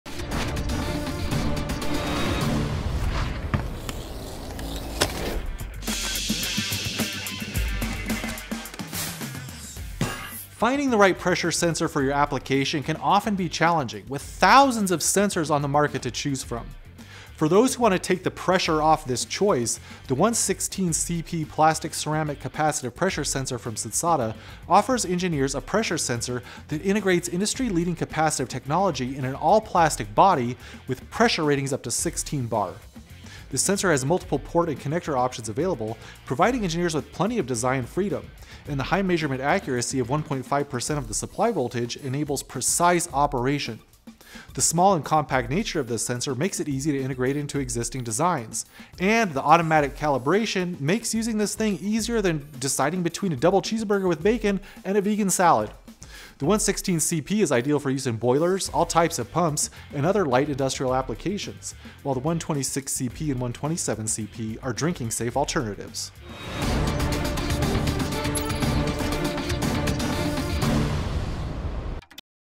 Sensata 116CP Plastic Ceramic Capacitive Pressure Sensor, Tech Specs | TTI, Inc.
Sensata 116CP Ceramic Capacitive Pressure Sensors are housed in a small and innovative plastic package ideal for boilers, pumps, and other light industrial applications. Sensata 116CP sensors are offered in a wide range of configurations, including various pressure ports, electrical outputs, and pressure ranges from 0-4bar to 0-16bar. These compact pressure sensors feature high accuracy of ±1.5%VS.

Follow TTI on LinkedIn for supply chain news and much more.